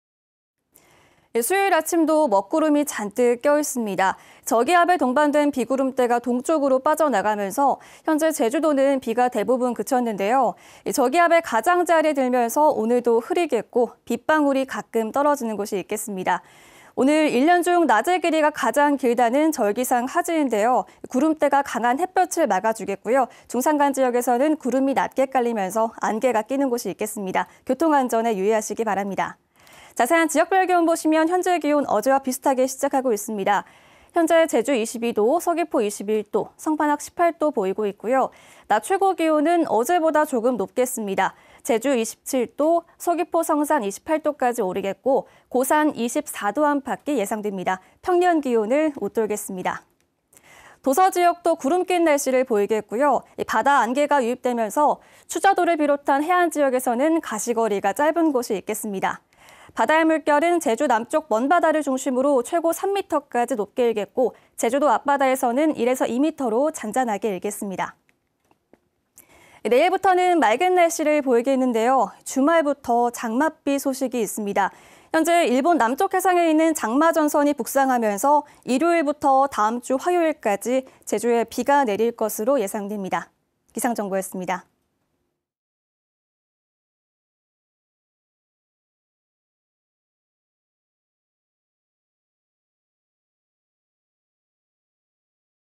절기'하지' 흐리고 가끔 빗방울‥일요일 '장맛비'
카카오톡 제보 : 제주MBC